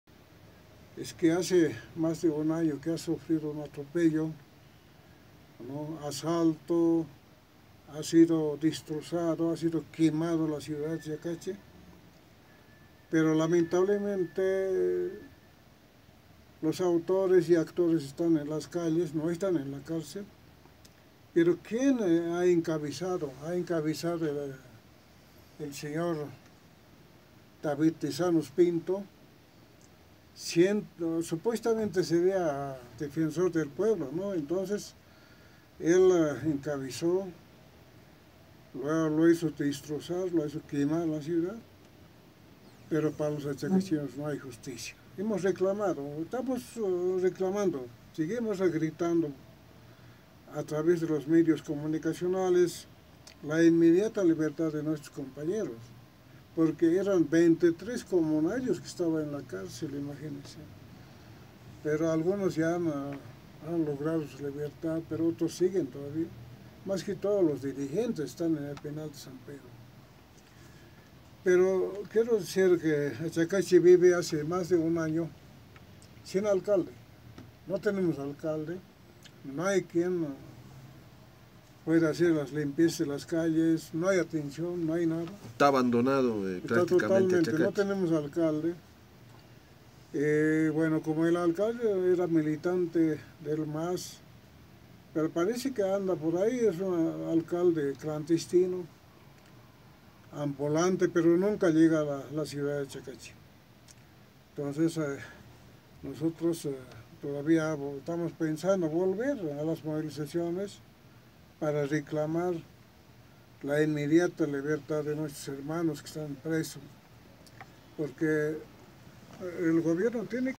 FELIPE QUISPE HUANCA EXIGE LIBERTAD PARA LOS PRESOS DE ACHACAHI
El ex ejecutivo de la CSUTCB recuerda el levantamiento popular en el municipio de Achacachi contra el alcalde del MAS, Eugenio Rojas. El mítico líder aymara exige libertad para los presos políticos.